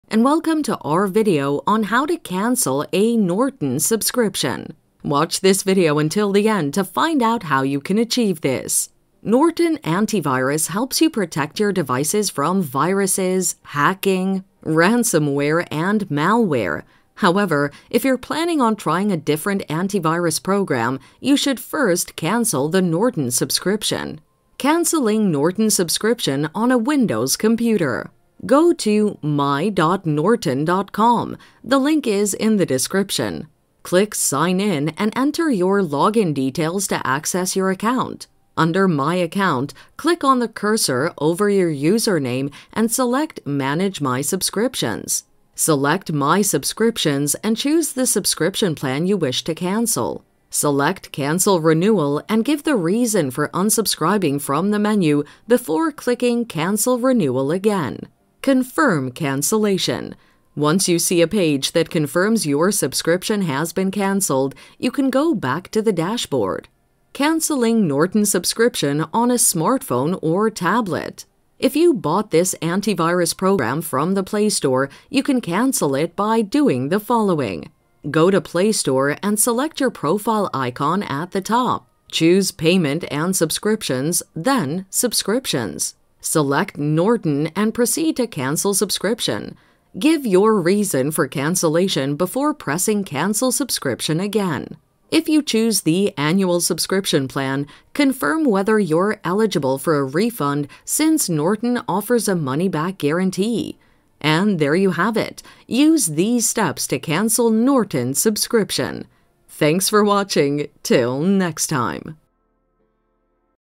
How To Cancel Norton Subscription (How To Stop Your Norton Subscription)
How To Cancel Norton Subscription (How To Stop Your Norton Subscription). In this video tutorial I will show how to cancel your Norton Subscription.

Norton Antivirus helps you protect your devices from viruses, hacking, ransomware, and malware. However, if you are planning on trying a different antivirus program, you should first cancel the Norton subscription.

Canceling Norton Subscription on a Windows computer
● Go to my.norton.com (the link is in the description).
● Click sign in and enter your login details to access your account.
● Under my account, click on the cursor over your username and select manage my subscriptions.
● Select my subscriptions and choose the subscription plan you wish to cancel.
● Select cancel renewal and give the reason for unsubscribing from the menu before clicking cancel renewal again.
● Confirm cancellation. Once you see a page that confirms your subscription has been canceled, you can go back to the dashboard.

Canceling Norton subscription on a smartphone or tablet
If you bought this antivirus program from the Play Store, you can cancel it by doing the following.
● Go to Playstore and select your profile icon at the top
● Choose payment and subscriptions, then subscriptions.
● Select Norton and proceed to cancel subscription.
● Give your reason for cancellation before pressing cancel subscription again.

If you chose the annual subscription plan, confirm whether you are eligible for a refund since Norton offers a money-back guarantee.